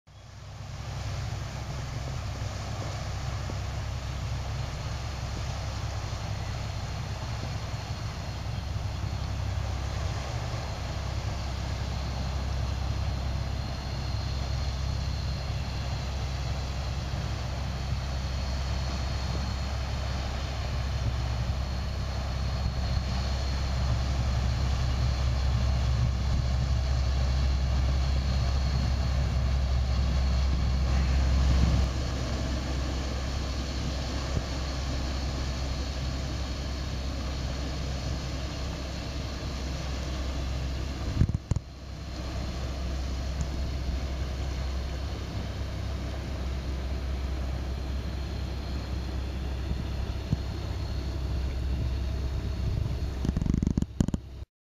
Une péniche ferrailleuse à Veneux-les-Sablons
Dimanche 1er juillet 2012, 18h20, passe une péniche remplie de déchets divers.